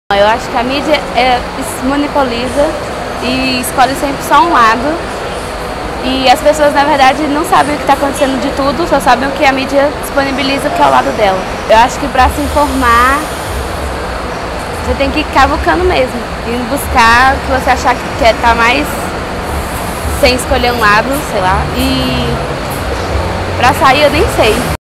De fora dos protestos: Estudante Bárbara Elisabeth não decidiu quem apoiar
Entrevistada pelo Portal EBC Barbara Elisabeth não vai participa dos protestos e afirma que  tem dificuldade em se informar pela mídia